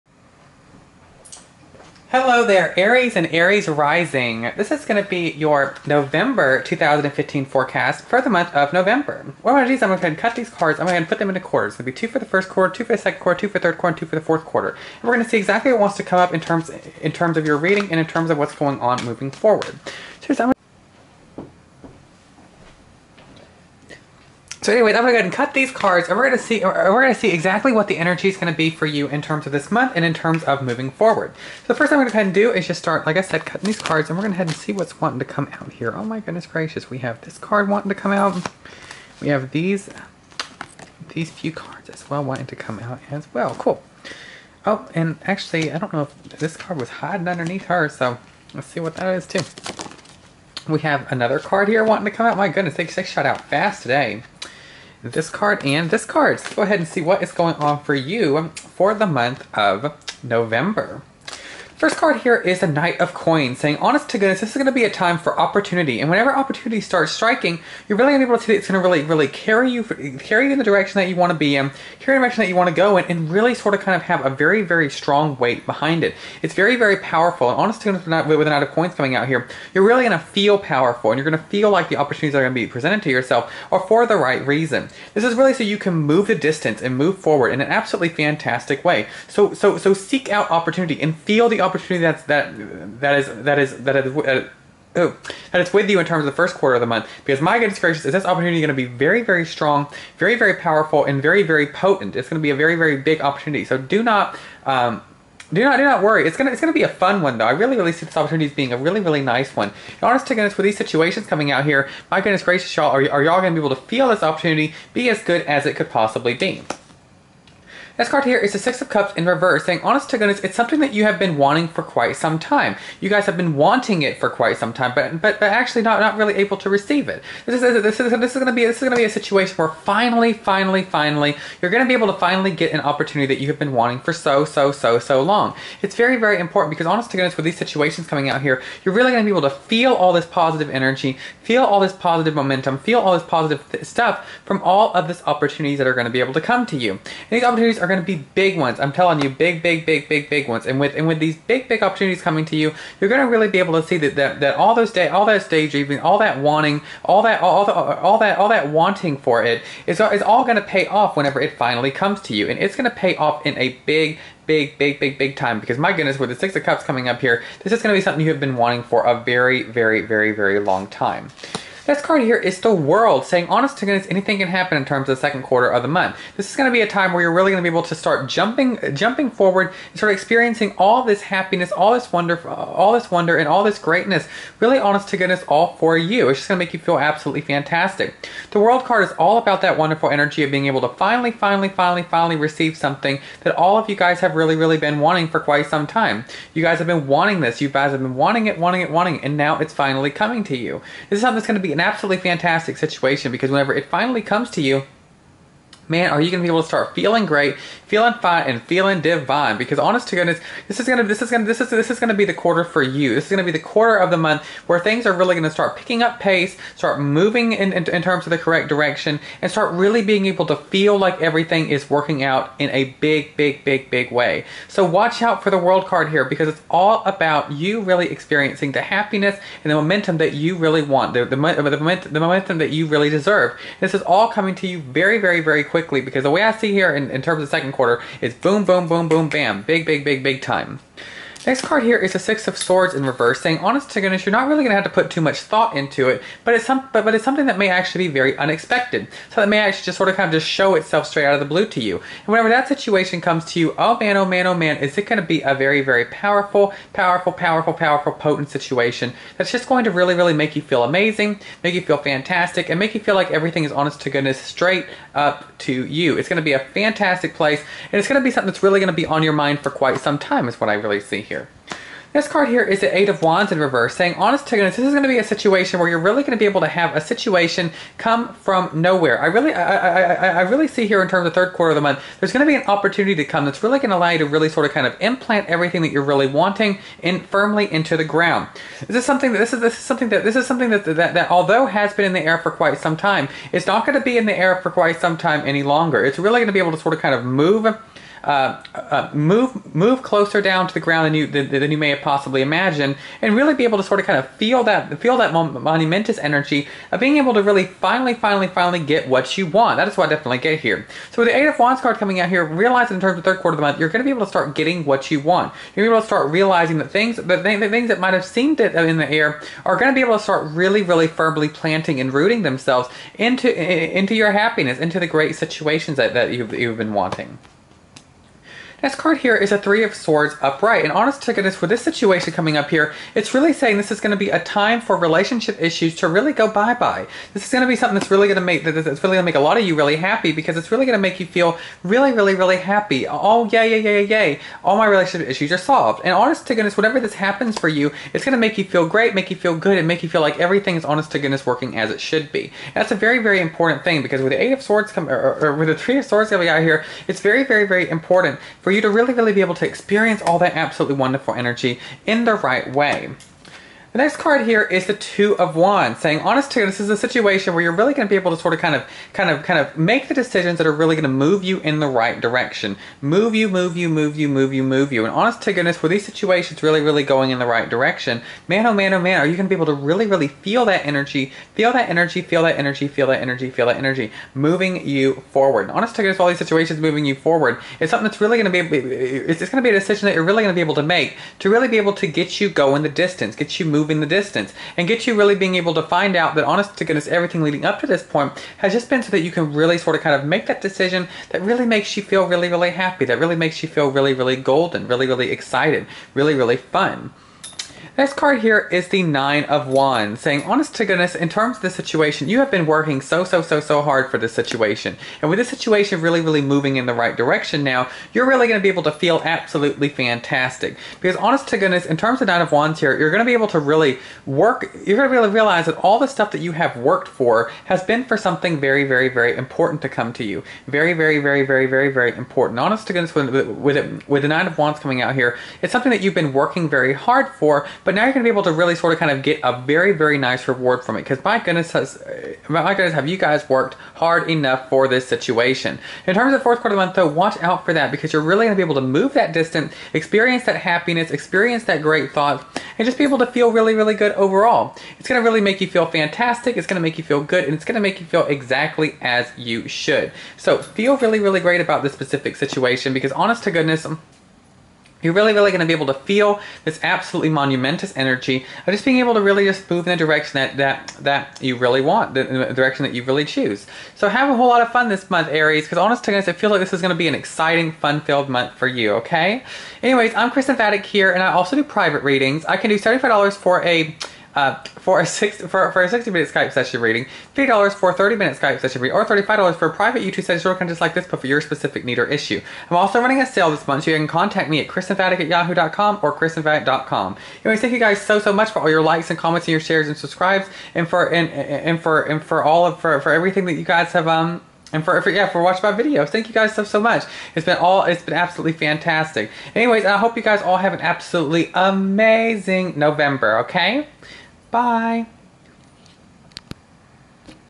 Aries November 2015 Monthly Intuitively Guided Tarot Reading
Hey there Aries and Aries Rising! This is your Intuitively Guided Monthly Tarot Forecast Reading for the month of November! :) I was very excited to post this one up~! Hope you guys have as much fun listening to it as I had making it! Thanks all!~